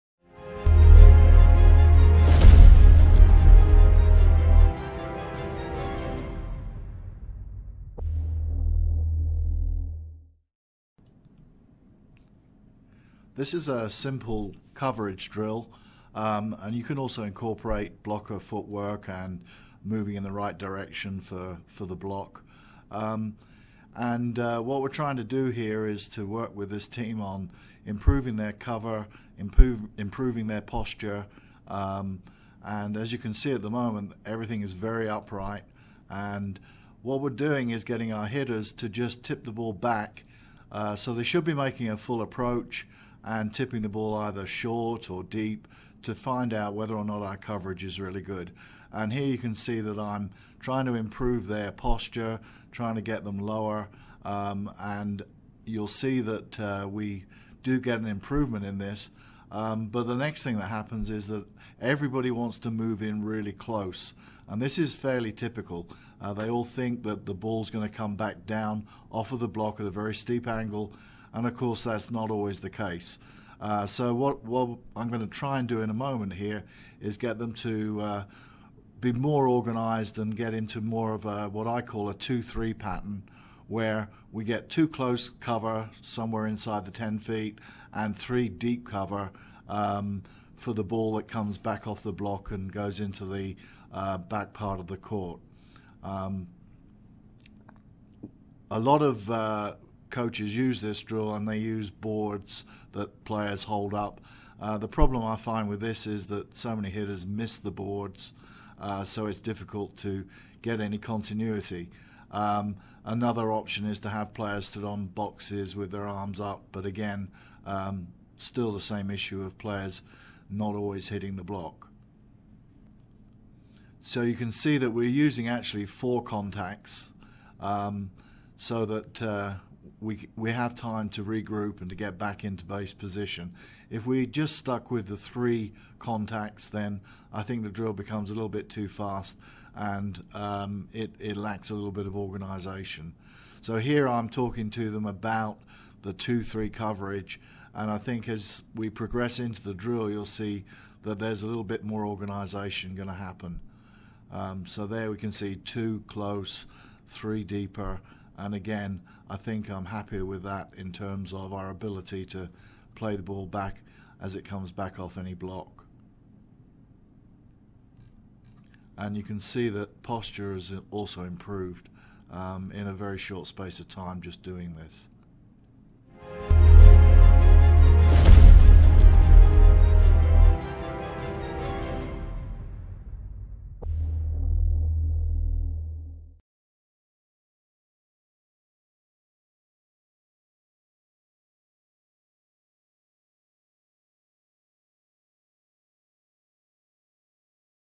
Volleyball Hitter Coverage Drill
Steve Colpus has his team going through a hitter coverage drill to work on his team covering his attackers for when they get blocked. As you can see in the drill the hitters are tipping the ball back to their own side and then they are digging the ball up and running another attack pattern.